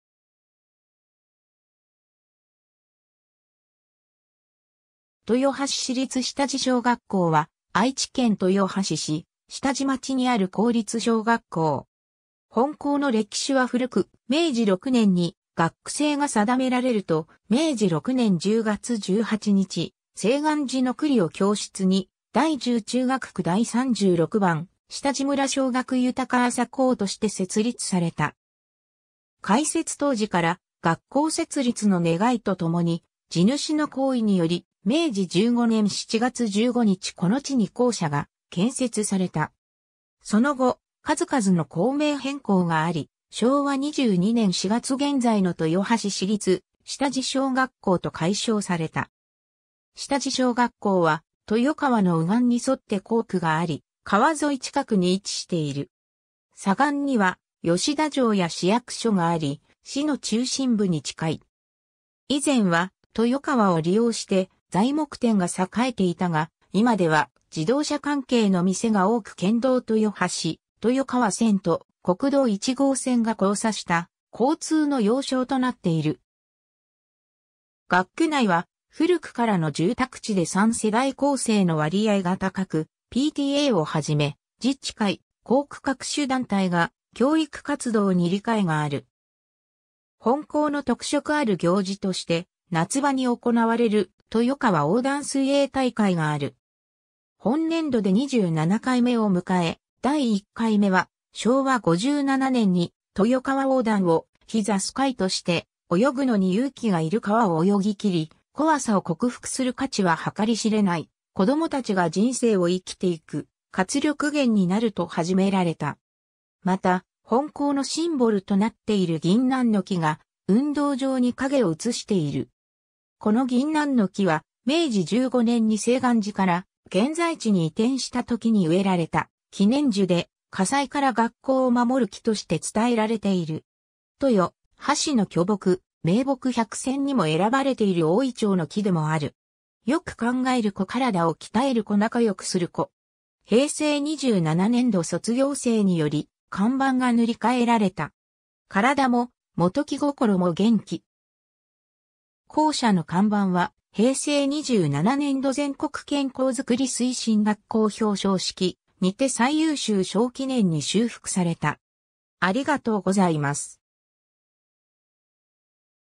豊橋市立下地小学校
豊橋市立下地小学校（とよはししりつしもじしょうがっこう）は愛知県豊橋市下地町にある公立小学校。
 本校の歴史は古く、明治6年に学区制が定められると、明治6年10月18日、聖眼寺の庫裡を教室に「第10中学区第36番下地村小学豊麻校」として設立された。
開設当時から学校設立の願いとともに、地主の好意により明治15年7月15日この地に校舎が建設された。
その後、数々の校名変更があり、昭和22年4月現在の豊橋市立下地小学校と改称された。
 下地小学校は、豊川の右岸に沿って校区があり、川沿い近くに位置している。
左岸には、吉田城や市役所があり、市の中心部に近い。
以前は、豊川を利用して材木店が栄えていたが、今では自動車関係の店が多く県道豊橋・豊川線と国道1号線が交差した交通の要衝となっている。
 学区内は、古くからの住宅地で三世代構成の割合が高く、PTAをはじめ、自治会・校区各種団体が教育活動に理解がある。
 本校の特色ある行事として夏場に行われる豊川横断水泳大会がある。
本年度で27回目を迎え、第1回目は、昭和57年に「豊川横断を日ざす会」として「泳ぐのに勇気がいる川を泳ぎ切り、怖さを克服する価値は計りしれない、子ども達が人生を生きていく活力源になる」と始められた。
 また、本校のシンボルとなっている銀杏の木が運動場に影を映している。
この銀杏の木は、明治15年に聖眼寺から現在地に移転した時に植えられた記念樹で、火災から学校を守る木として伝えられている。
「とよはしの巨木・名木百選」にも選ばれている大銀杏の木でもある。
 よく考える子体を鍛える子仲良くする子。
平成27年度卒業生により看板が塗り替えられた。
 体も元気心も元気。
校舎の看板は、平成27年度全国健康づくり推進学校表彰式にて最優秀賞記念に修復された。